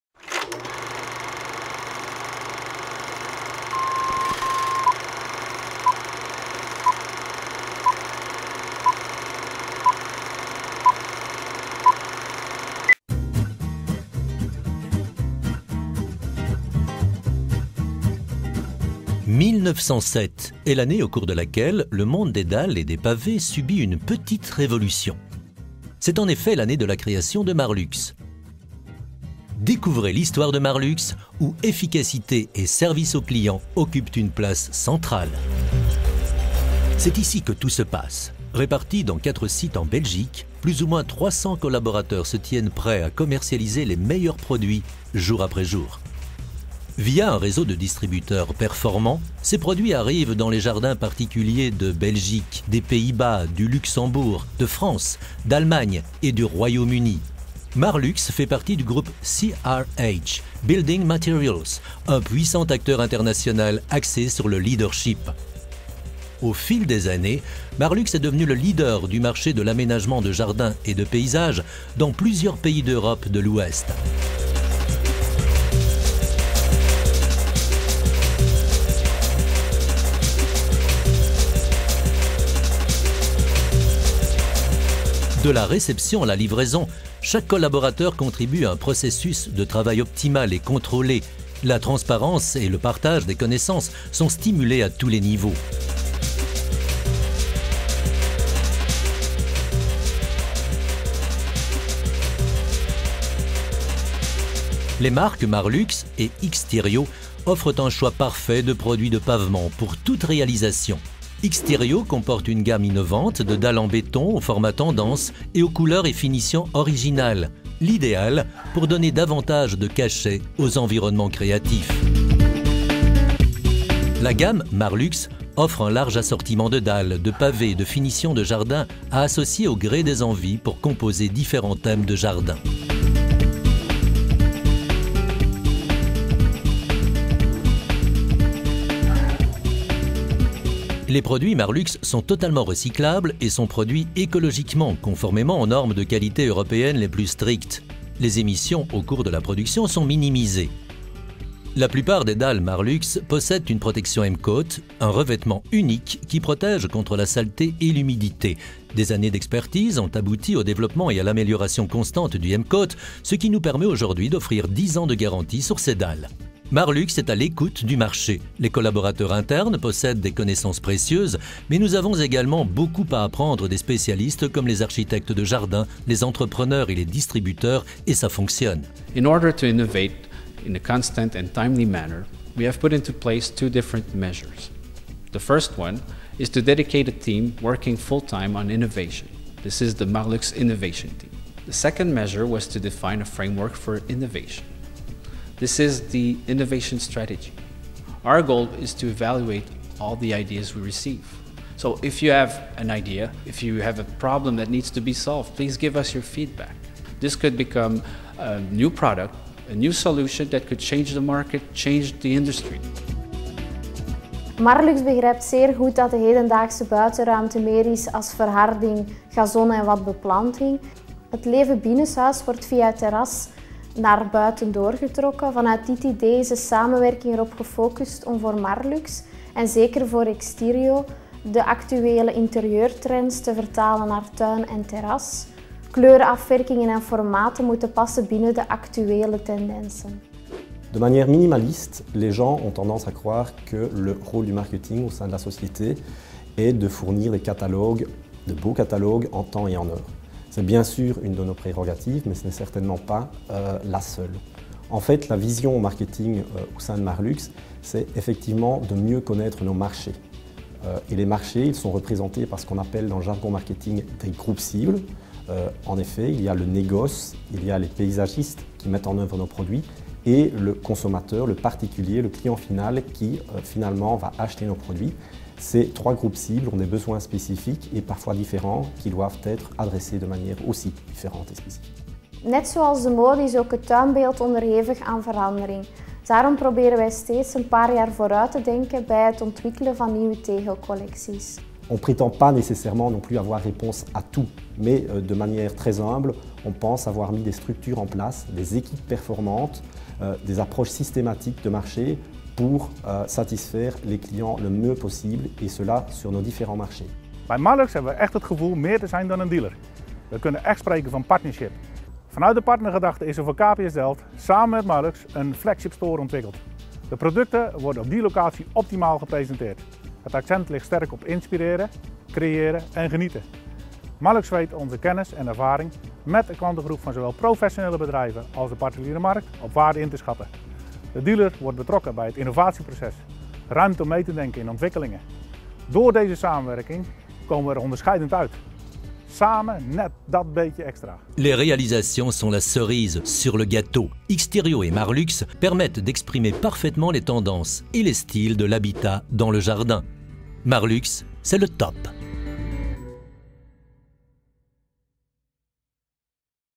Marlux dalles et pavés - Le film
Depuis 100 ans vous trouvez des dalles et pavés de qualité pour jardins, terrasses, balcons, allées, chemins et piscines chez Marlux.

Quel jardin vous correspond le mieux?

Curieux de savoir quel style correspond le mieux à votre patio?  Choisissez votre style